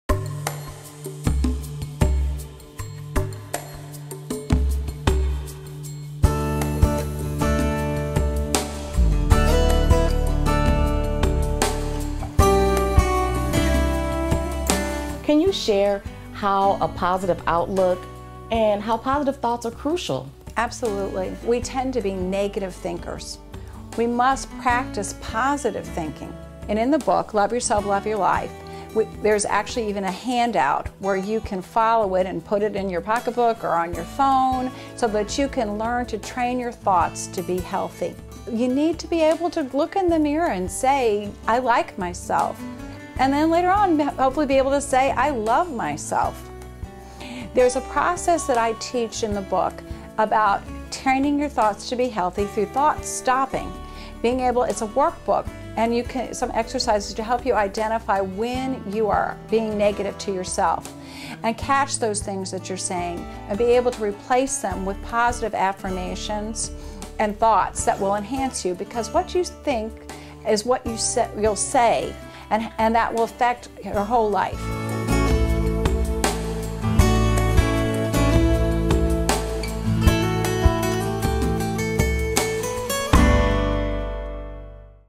Dr. Anne Creekmore Short 1 - Postive Thoughts are Crucial
This video is about Dr. Anne Creekmore's book, "Love Yourself, Love Your Life".